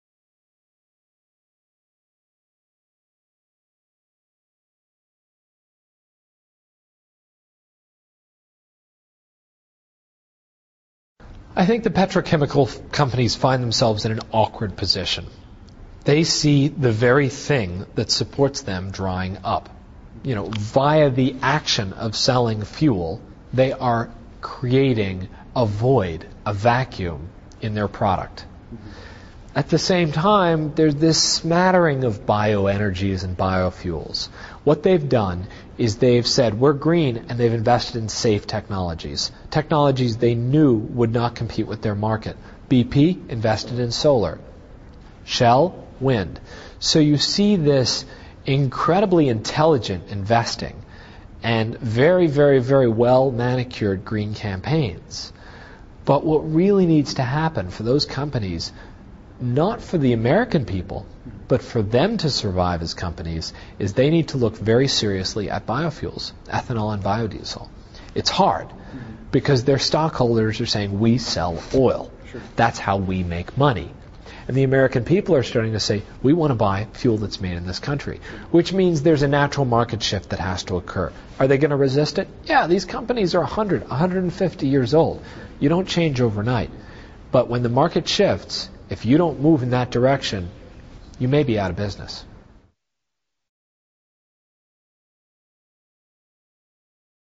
Interview: Big Oil - Josh Tickell Video
Author&nbsp;Josh Tickell discusses what the big oil companies think about bio-diesel